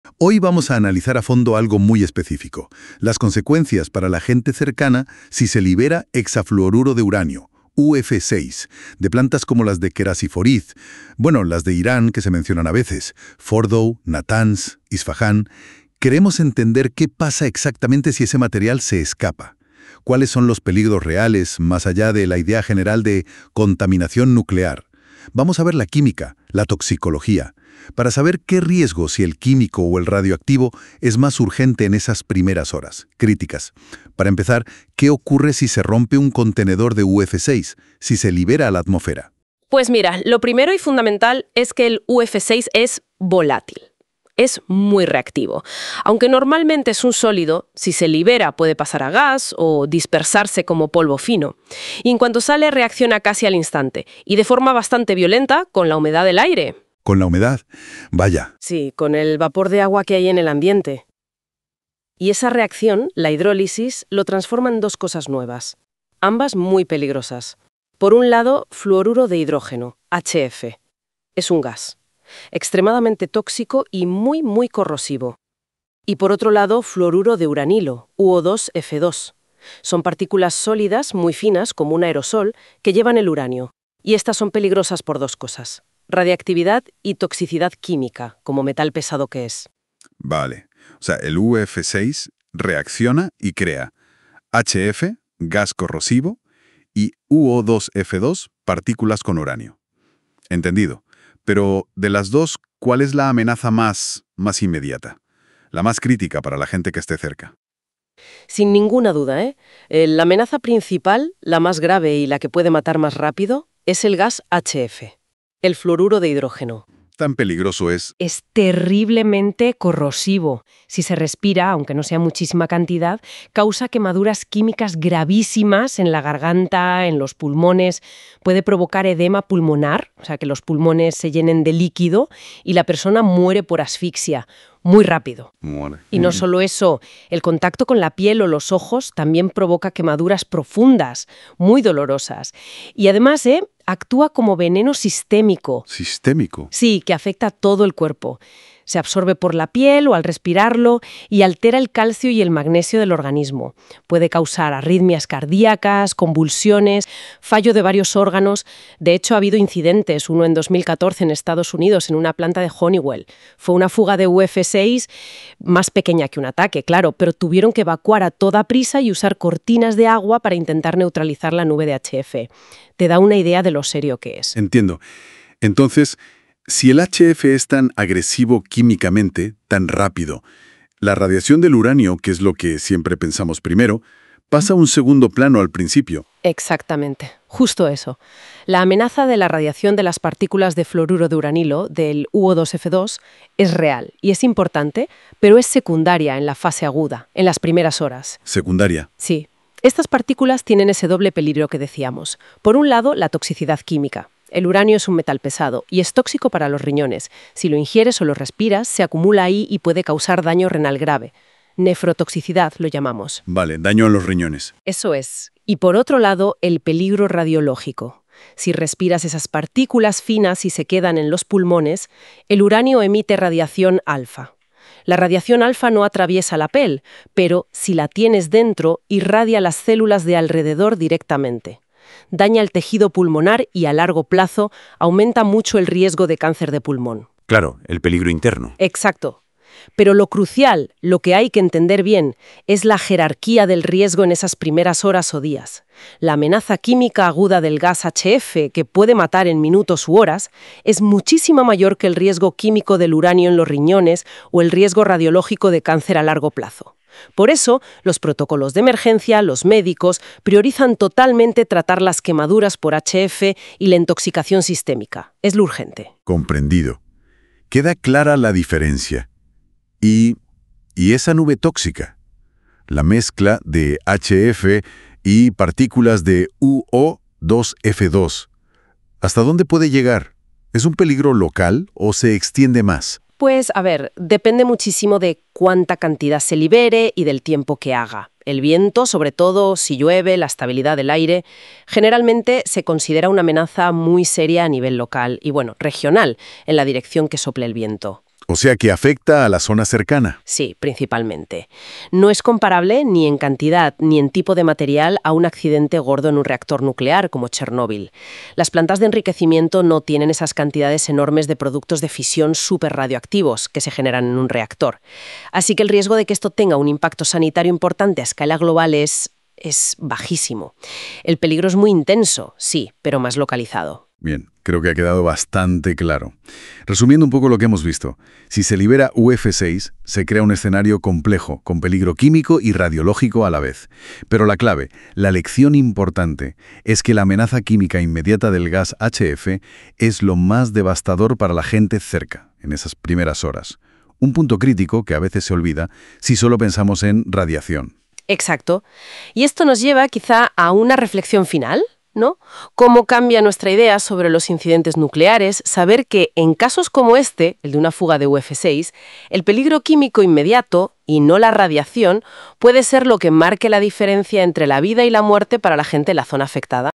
¡Riesgo Químico! Liberación de UF6 en Isfahán, Natanz y Fordow después de ataques.
El ataque a las salas de centrifugadoras en Natanz y Fordow, o a los cilindros de almacenamiento de UF6 en Isfahán, probablemente haya producido la liberación de este material a la atmósfera y con total seguridad se habrá dispersado por las instalaciones subterráneas.
El hexafluoruro de uranio (UF6) es un compuesto único que, aunque es un sólido cristalino a temperatura y presión ambiente, sublima fácilmente a estado gaseoso a temperaturas ligeramente elevadas o cuando se libera de la presión de sus contenedores.
El UF6 es un compuesto extremadamente reactivo, particularmente con el agua. Al ser liberado en la atmósfera, incluso con la humedad ambiental normal, sufre una reacción de hidrólisis violenta y exotérmica. Esta reacción transforma instantáneamente el UF6  en dos subproductos principales, ambos altamente peligrosos:
Fluoruro de Uranilo (UO2F2): Un compuesto sólido que se forma como un aerosol de partículas finas, a menudo descrito como un polvo amarillento. Este compuesto es el portador del átomo de uranio y, por lo tanto, es radiactivo y un metal pesado tóxico.
Las partículas de aerosol de UO2F2 contienen el isótopo U−235 enriquecido al 60%. El U−235 es un emisor de partículas alfa. La radiación alfa tiene un alcance muy corto y no puede penetrar la capa externa de la piel, por lo que no representa un peligro externo. Sin embargo, si una partícula emisora de alfa es inhalada o ingerida, se convierte en un peligro interno extremadamente grave. Una vez depositada en los pulmones o en otros órganos, la partícula irradia las células circundantes a corta distancia, causando un daño celular que puede conducir al desarrollo de cáncer, especialmente cáncer de pulmón, a lo largo de los años.
Independientemente de su radiactividad, el uranio es un metal pesado tóxico. Su principal órgano diana en el cuerpo humano es el riñón. El uranio que se inhala o ingiere y entra en el torrente sanguíneo es filtrado por los riñones. Allí puede acumularse, causando daño químico a los túbulos renales. Esto puede llevar a una disfunción renal, que puede ser grave y, en casos de alta exposición, permanente (nefrotoxicidad).
El segundo subproducto de la hidrólisis del UF6 es el fluoruro de Hidrógeno (HF): Un gas extremadamente tóxico y corrosivo. Cuando se disuelve en agua, forma ácido fluorhídrico, una de las sustancias más corrosivas conocidas.
El peligro más inmediato y potencialmente mortal para cualquier población situada en la dirección del viento desde las instalaciones destruidas sería la exposición a la pluma de gas de fluoruro de hidrógeno (HF).
El HF al ser inhalado, ataca  las membranas mucosas de la nariz, la garganta y los pulmones, provocando quemaduras químicas graves que conducen a un edema pulmonar (acumulación de líquido en los pulmones), lo que puede causar la muerte por asfixia en cuestión de minutos u horas. El contacto con la piel, incluso en forma de gas, provoca quemaduras profundas y extremadamente dolorosas que pueden no ser evidentes de inmediato, ya que el gas penetra en los tejidos antes de causar daños visibles.
Más allá de su efecto corrosivo local, el HF es un veneno sistémico. Los iones de flúor se absorben rápidamente en el torrente sanguíneo, donde se unen al calcio y al magnesio del cuerpo. Esto provoca una grave alteración del equilibrio electrolítico conocida como hipocalcemia, que puede desencadenar arritmias cardíacas, convulsiones y la muerte.
Las poblaciones cercanas a las plantas destruidas pueden estar en grave riesgo, siendo el HF la mayor amenaza en este momento, mientras la contaminación radiológica mostrará sus efectos en las próximas décadas.